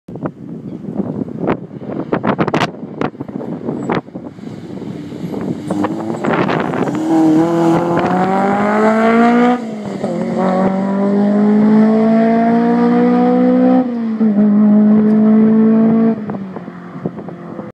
Toyota MR-S K20A
250bhp, 1033kg (50/50 weight balance), DC5 K20A engine Jdm Transplant.